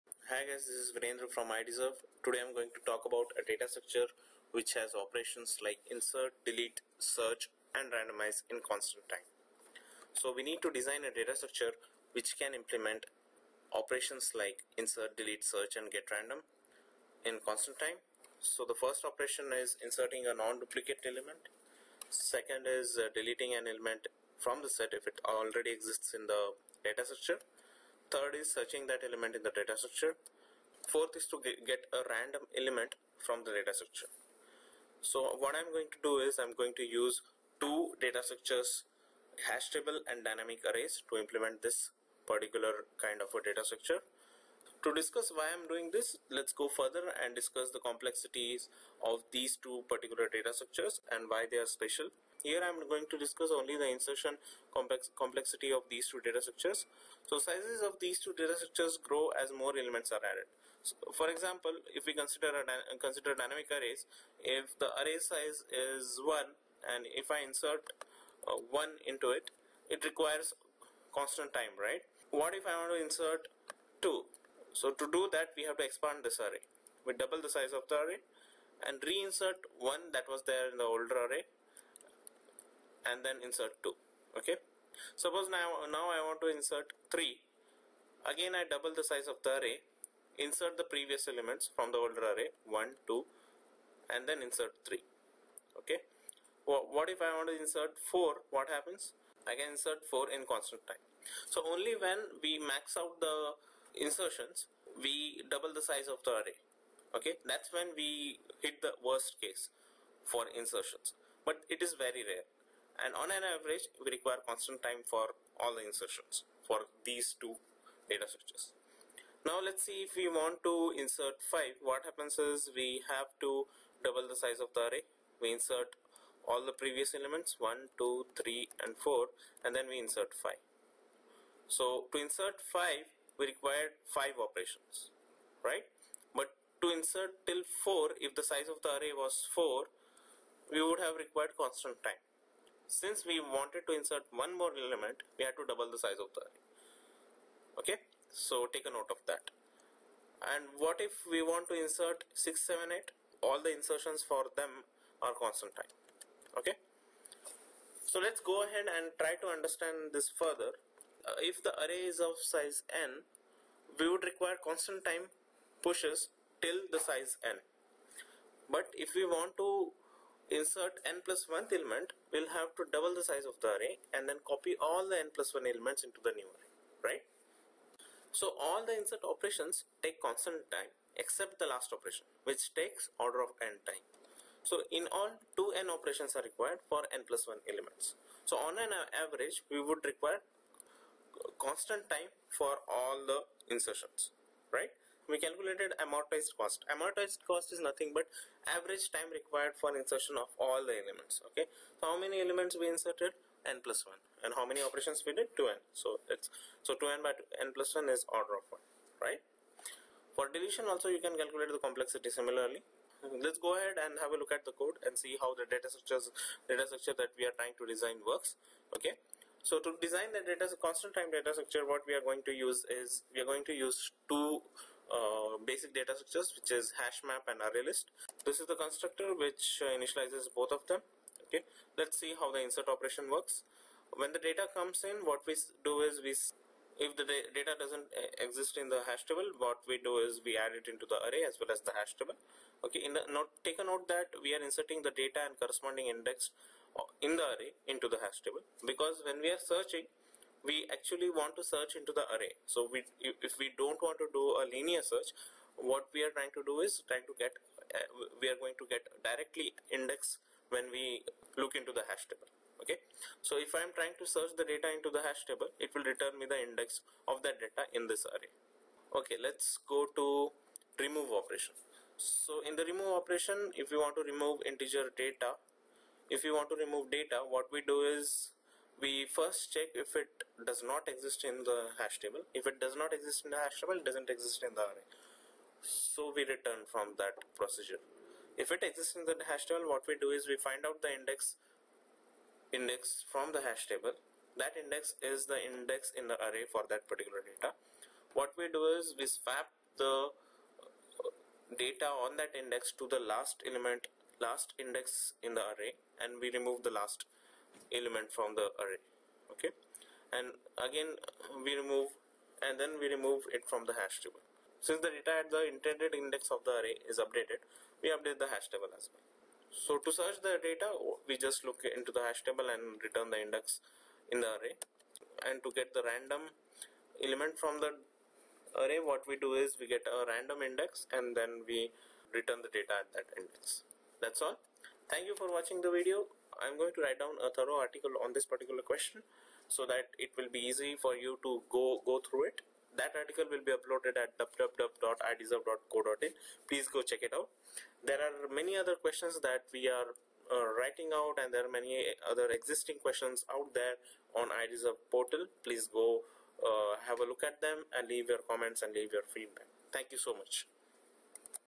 Design a data structure with operations Insert, Delete, Search and GetRandom in Constant Time
Design Datastructure Insert Delete Search GetRandom Constant Time

This video explains the Amortized Analysis of HashMaps and ArrayLists which are used in designing the constant time data-structure.